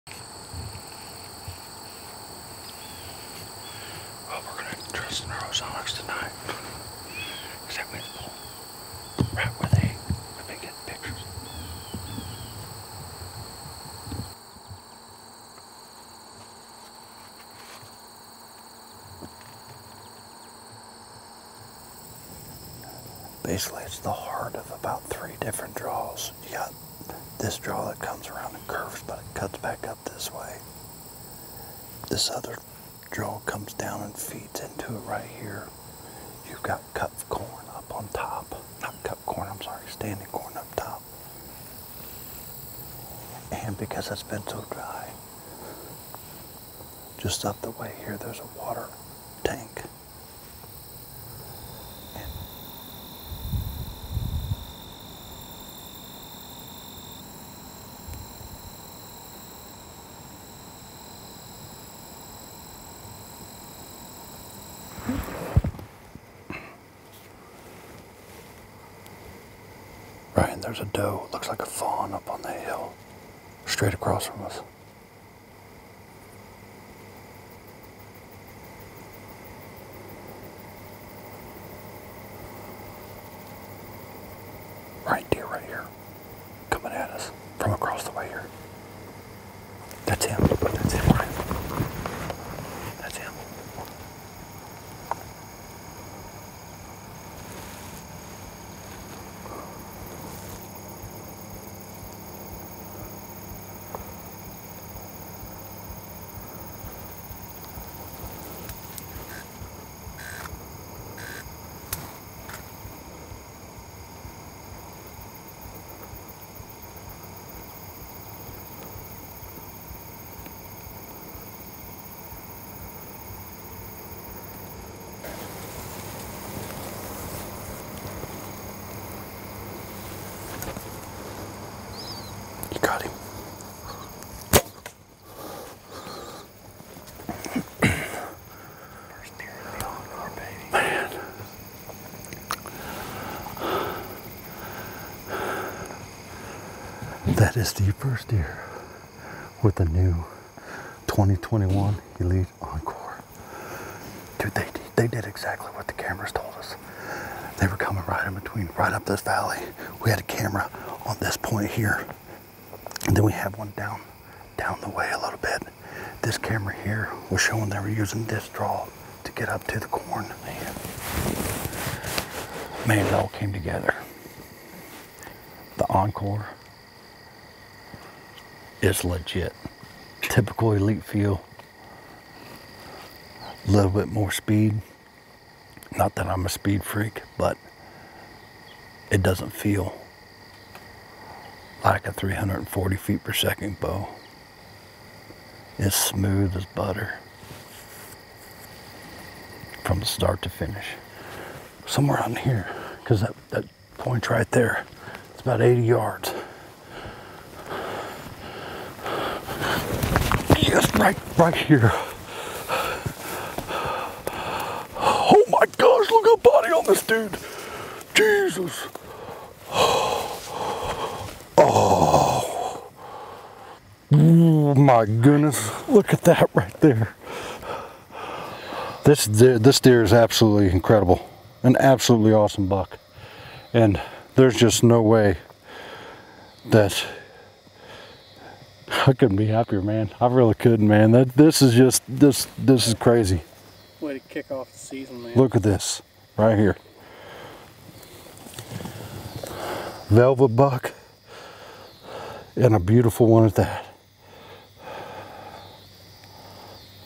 VELVET BOONER DOWN!! The 2021 Elite ENKORE goes to WORK.
Larry McCoy of Respect The Game knocks down a giant Nebraska buck with the brand new 2021 Elite ENKORE.   Check out the hunt then go shoot the EnKore at your local retailer.